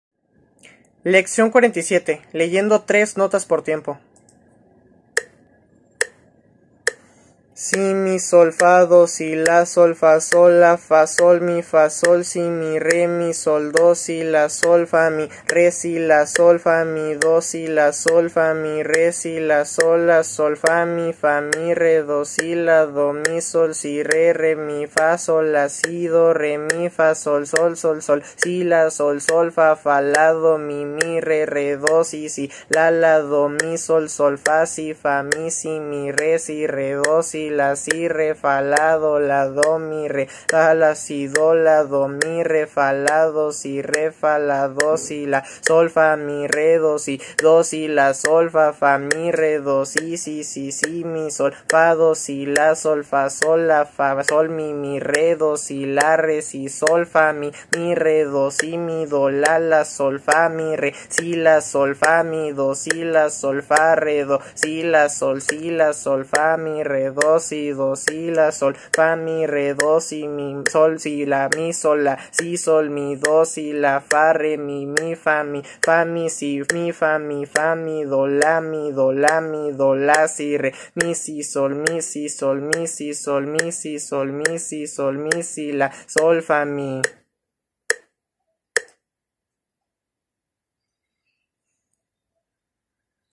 Solfeo y Entrenamiento Auditivo VI: Lección #47 (Leyendo tres notas por tiempo)
Video de Nathán Cuadros